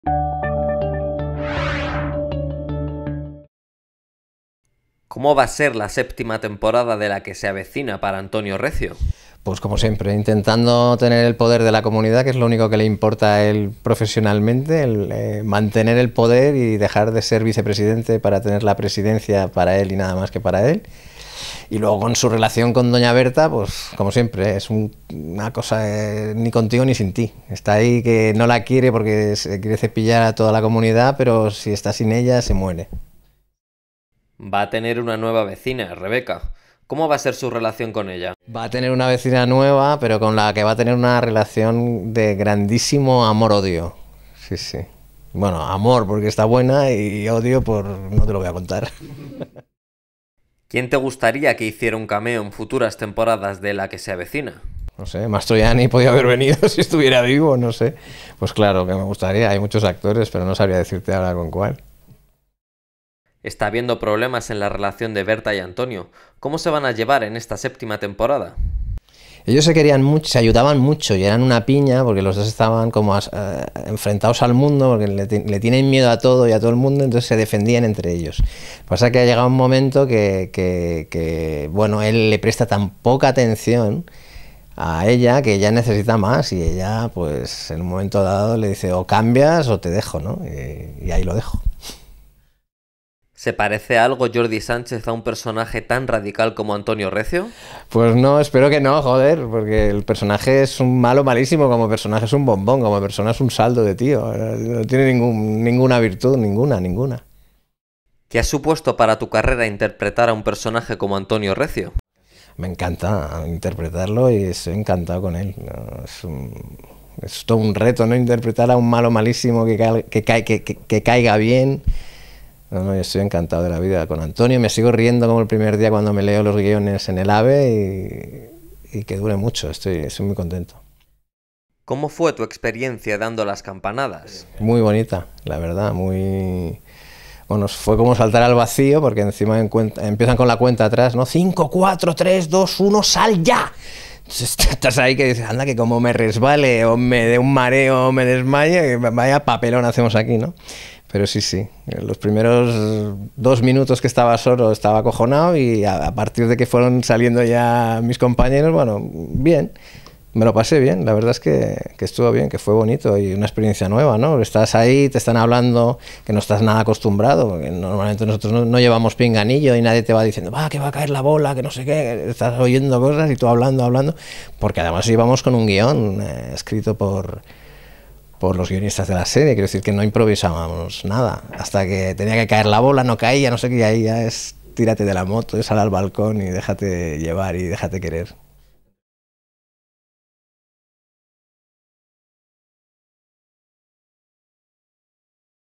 Jordi Sánchez: "Como personaje, Antonio Recio es un bombón. Como persona es un saldo de tío"
Adelanta también que su relación con Rebeca será de "amor-odio" en la séptima temporada de 'La que se avecina'.